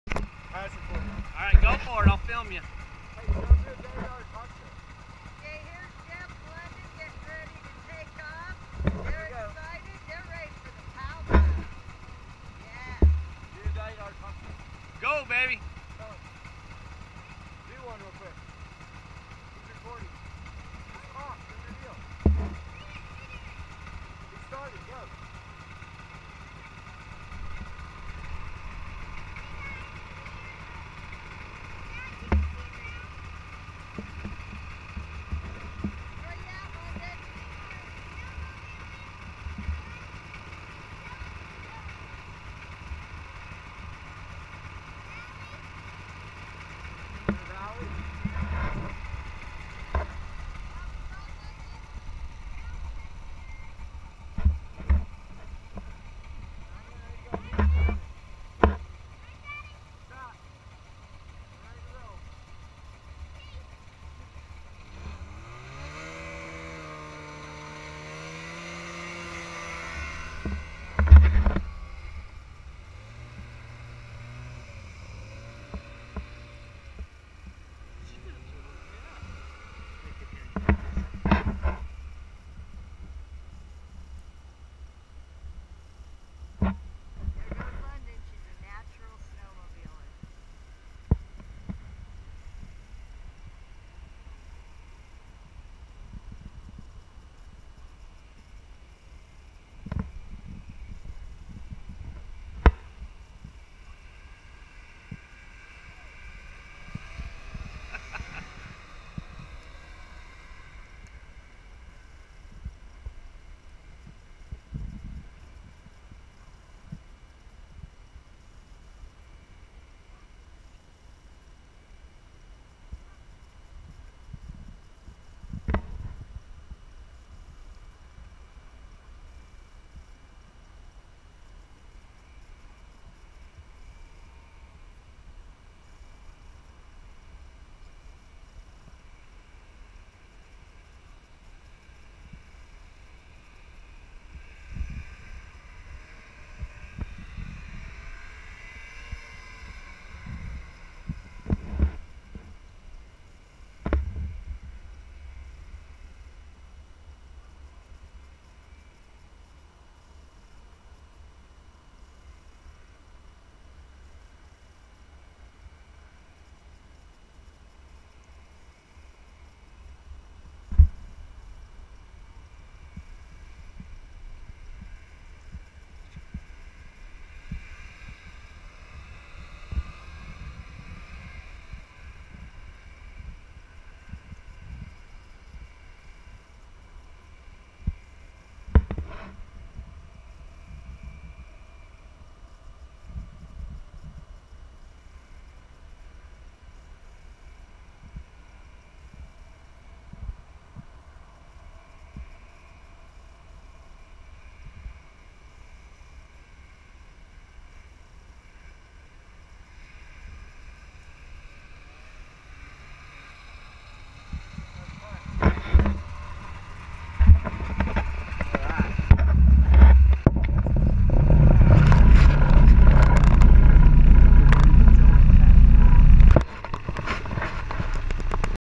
Snowmobiling with London
Fun Times in Eagle CO with London Johnston